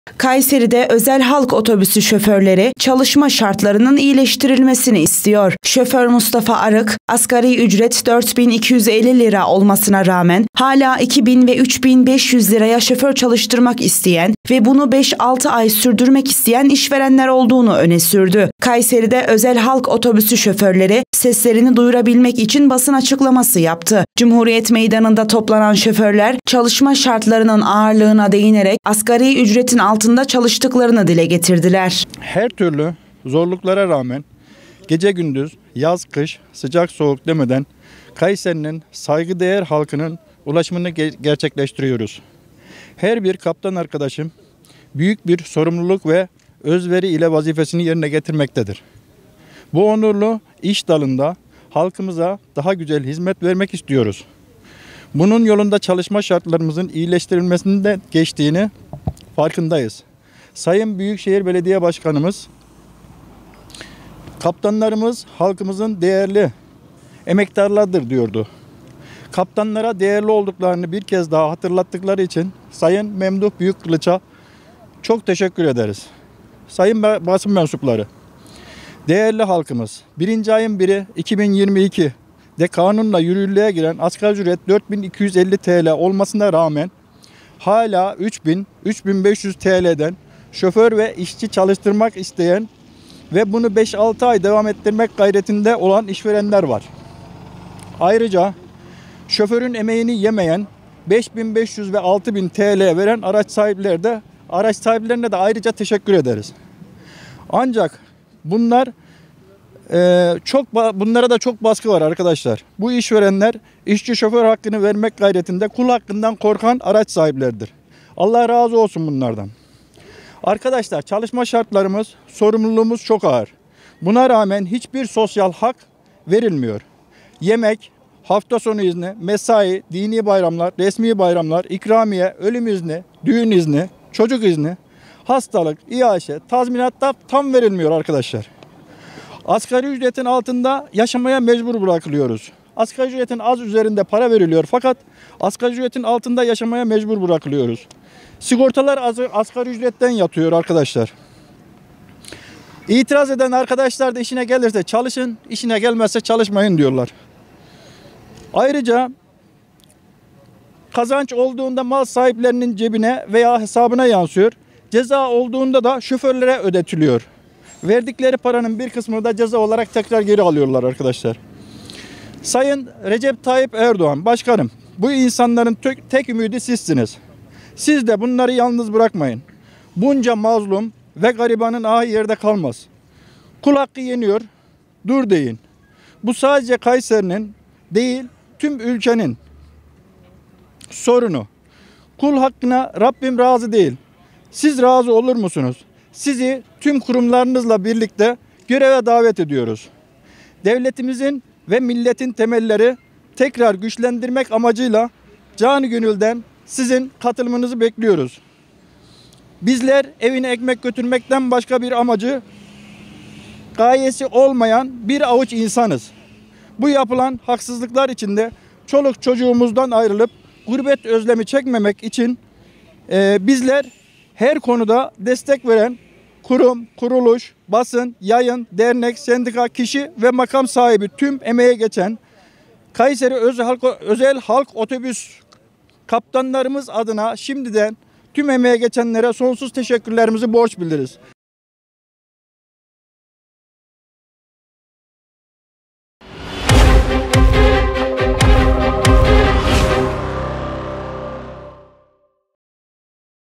Özel halk otobüsü şoförleri çalışma şartlarının iyileştirilmesini istiyor
KAYSERİ, (BGC-ÖZEL HABER)- Kayseri'de özel halk otobüsü şoförleri, çalışma şartlarının iyileştirilmesini istiyor. Şoför Mustafa Arık, asgari ücret 4 bin 250 lira olmasına rağmen hala 2 bin ve 3 bin 500 liraya şoför çalıştırmak isteyen ve bunu 5-6 ay sürdürmek isteyen işverenler olduğunu öne sürdü.

Kayseri'de Özel Halk Otobüsü şoförleri, seslerini duyurabilmek için basın açıklaması yaptı. Cumhuriyet Meydanı’nda toplanan şoförler, çalışma şartlarının ağırlığına değinerek asgari ücretin altında çalıştıklarını dile getirdiler.

Özel Halk Otobüsü Şoförü Mustafa Arık, "Biz Özel Halk Otobüsü Kaptanları olarak her türlü zorluklara rağmen gece gündüz, yaz kış, sıcak soğuk demeden Kayseri halkının ulaşımını gerçekleştiriyoruz. Biz halkımıza daha iyi hizmet vermek istiyoruz. Bunun yolu da çalışma şartlarının iyileştirilmesinden geçmektedir. Büyükşehir Belediye Başkanının dediği gibi kaptanlar halkın değerli emektarlarıdır. 1 Ocak 2022 tarihinden itibaren yürürlüğe giren Asgari Ücret 4 bin 250 lira olmasına rağmen hala 2 bin ve 3 bin 500 liradan şoför çalıştırmak isteyen ve bunu 5-6 ay devam ettirmek isteyen işverenler var" diye konuştu.

"SOSYAL HAK VERİLMİYOR "

Zor şartlarda çalıştıklarını belirten Arık, buna rağmen sosyal hak verilmediğini öne sürdü. Arık, "Yemek, hafta sonu izni, mesai, dini bayramlar, resmi tatiller,  ikramiye, ölüm izni, doğum izni, çocuk izni, hastalık, iaşe ve tazminat tam verilmiyor. Devletin kanunlarına karşı geliyorlar. Asgari ücretin biraz üzerinde maaş verip, Asgari ücretin altında yaşamaya mecbur bırakıyorlar. Sigortalar asgari ücretten yatırılıyor. İtiraz ettiğin takdirde bir işe yaramıyor. İşine gelirse böyle' diyorlar. Kazanç olursa araç sahiplerinin hesabına aktarılıyor. Ceza yazıldığında şoförden alınıyor. Bizler evine ekmek götürmekten başka amacı olmayan insanlarız" dedi.